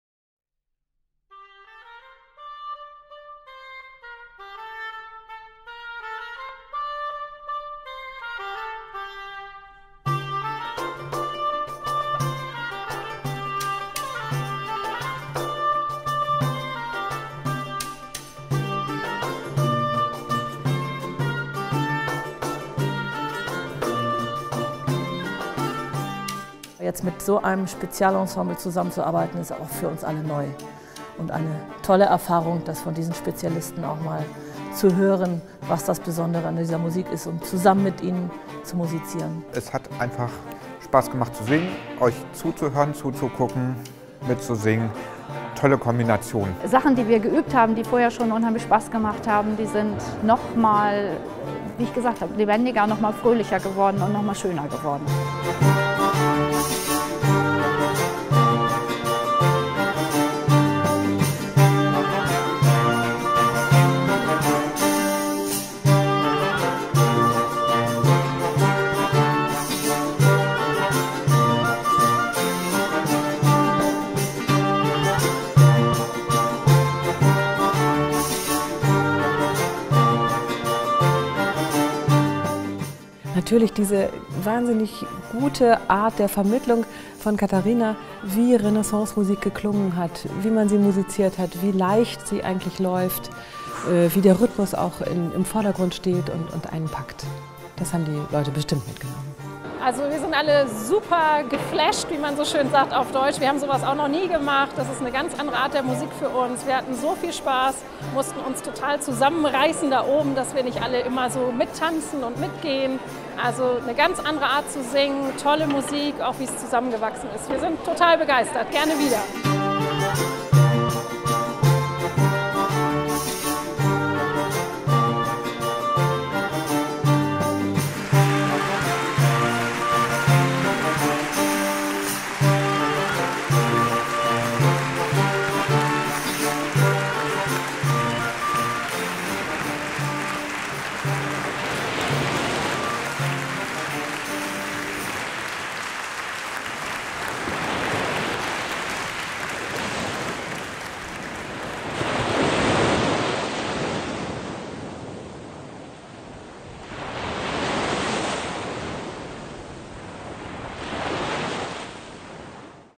EIN FEST FÜR MARTIN LUTHER - Teaser
Landeskirchenweite Workshop- und Konzertreihe
für Kirchenchöre und CAPELLA DE LA TORRE

Zum 500-jährigen Reformationsjubiläum stellte die Ev.-luth. Landeskirche Hannovers das vokale und instrumentale Musizieren zu Luthers Zeit in den Mittelpunkt einer besonderen, landeskirchenweiten Konzertreihe: Gemeinsam mit den örtlichen Kirchenchören präsentierte das Spezialisten-Ensemble für Alte Musik CAPELLA DE LA TORRE (ECHO KLASSIK-Preisträger 2016 und 2017) an 13 Spielorten in Niedersachsen Musik aus der Renaissance.

Idee & Konzept: VISION KIRCHENMUSIK
Film: Anna-Kristina Bauer

EIN FEST FÜR MARTIN LUTHER wurde gefördert von der Ev.-luth. Landeskirche Hannovers und der Niedersächsischen Sparkassenstiftung.